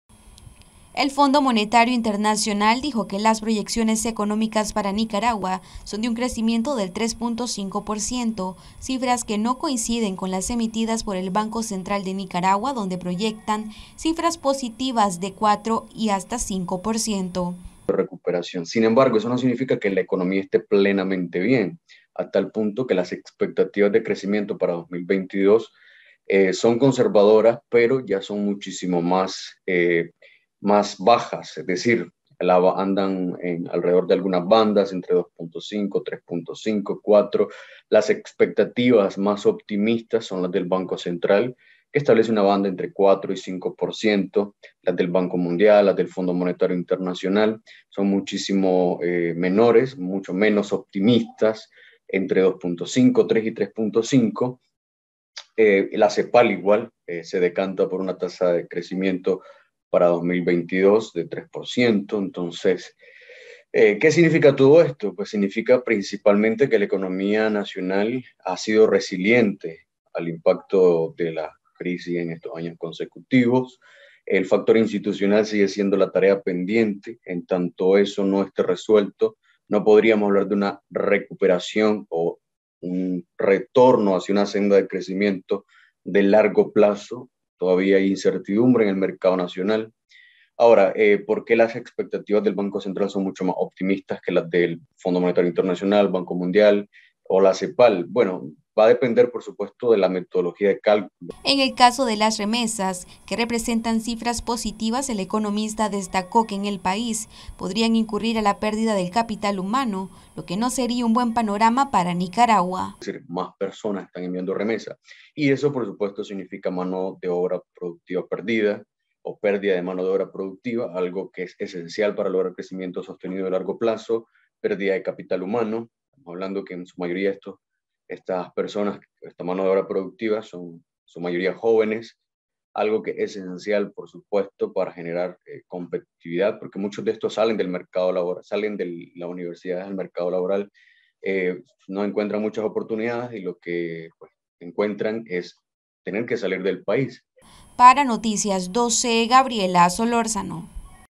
Proyecciones internacionales sobre la economía del país son conservadores
Noticias12Nicaragua Proyecciones internacionales sobre la economía del país son conservadores, asegura economista independiente